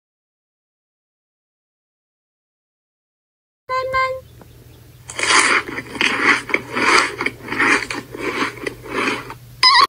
Sun eats a lemon || Sun and moon show || Flipaclip Animatic
@SunMoonShow  sun eats a lemon for the first time! (I think-)
@AminaTVdemon  I got the inspiration of trying out flipaclip from you :D
Also please understand I did not try to steal your art style in any way shape or form- I now
It doesn’t look like I did but still just saying! Also I love your videos!